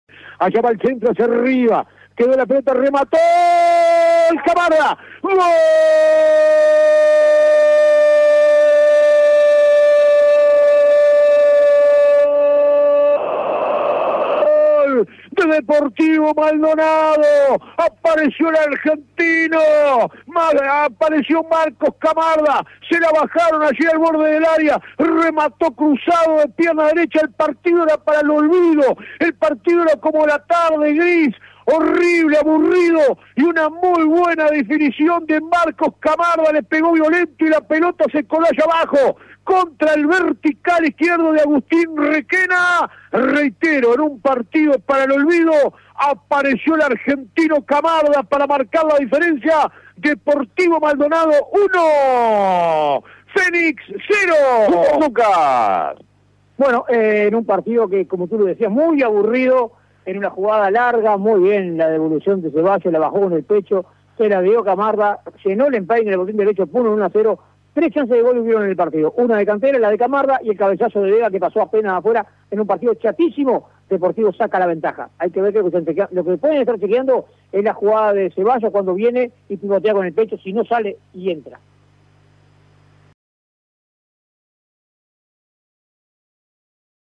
Fénix 0 Deportivo Maldonado 1 - Clausura 2022 (por Román Nappa)
El gol del partido, por Román Nappa para Cadena del Mar de Maldonado.